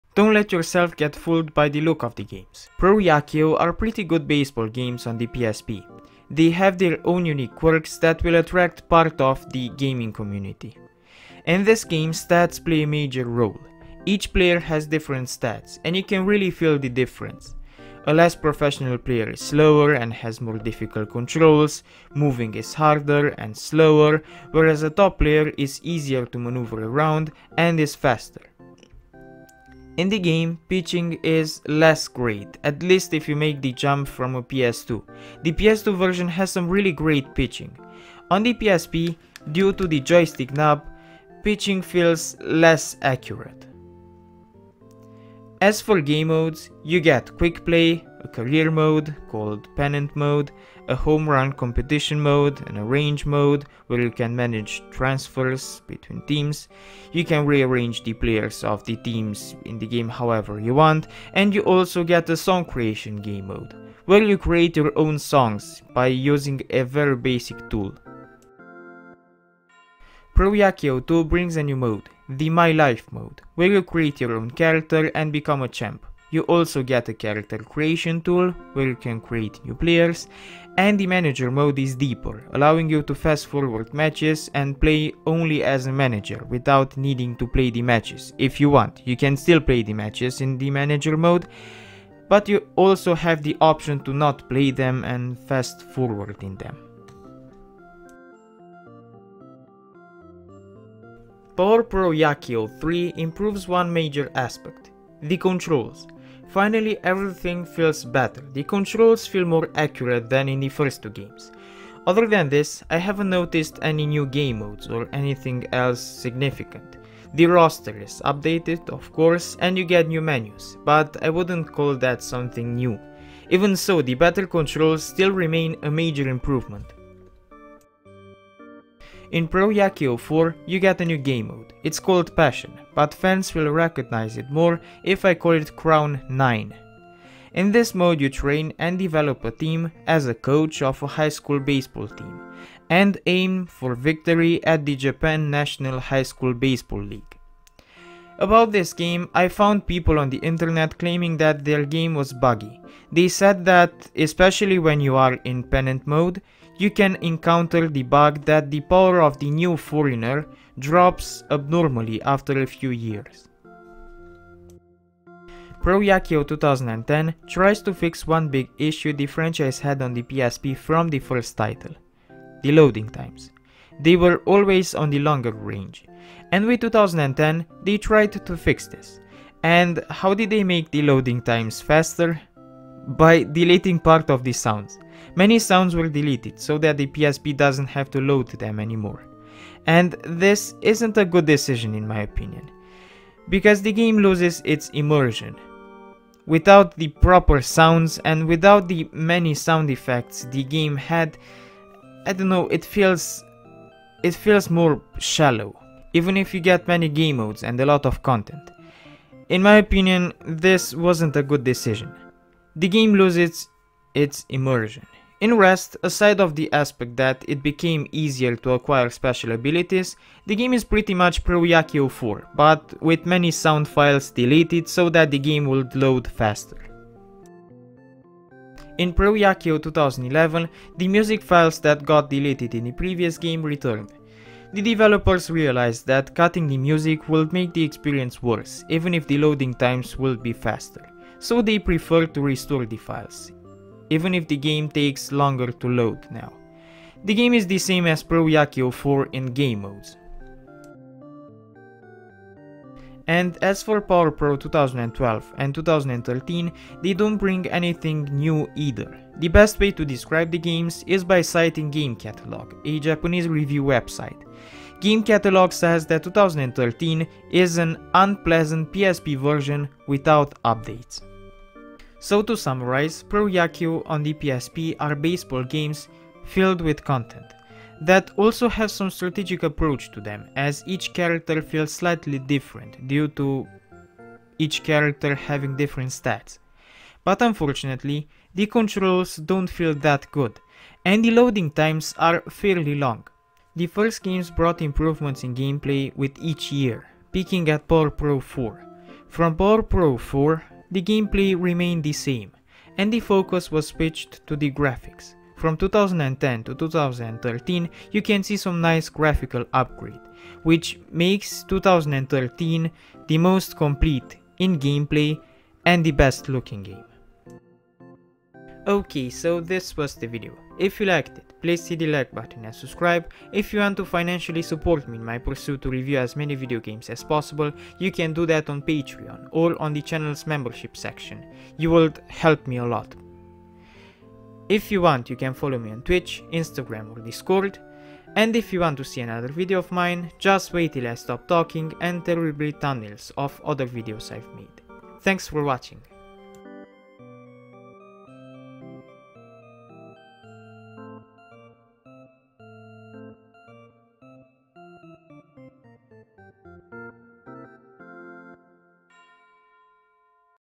All Jikkyou Powerful Pro Yakyuu Portable Games for PSP Review
Here is a list of all Jikkyou Powerful Pro Yakyuu Portable Games for  the SONY PSP:
1.  Jikkyou Powerful Pro Yakyuu Portable (2006)
2.  Jikkyou Powerful Pro Yakyuu Portable 2 (2007)
3.  Jikkyou Powerful Pro Yakyuu Portable 3 (2008)
4.  Jikkyou Powerful Pro Yakyuu Portable 4 (2009)
5.  Jikkyou Powerful Pro Yakyuu Portable 2010
6.  Jikkyou Powerful Pro Yakyuu Portable 2011
7.  Jikkyou Powerful Pro Yakyuu Portable 2012
8.  Jikkyou Powerful Pro Yakyuu Portable 2013